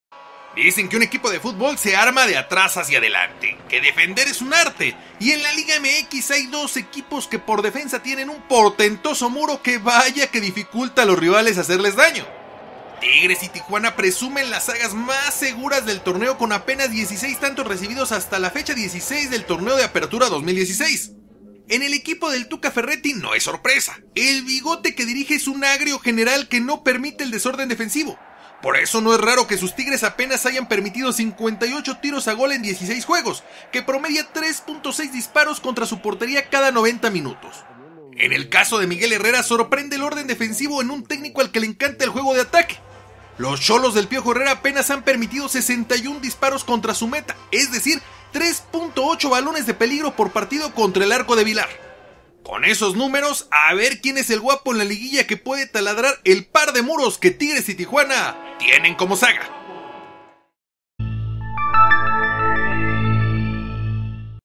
Tigres y Tijuana; y el arte de la defensa
APERTURA 2016. Ciudad de México. 2016/11/15. Dicen que un equipo de futbol se arma de atrás hacia adelante. Que defender es un arte; y en la Liga MX hay dos equipos que por defensa tienen un portentoso muro que vaya que dificulta a los rivales hacerles daño.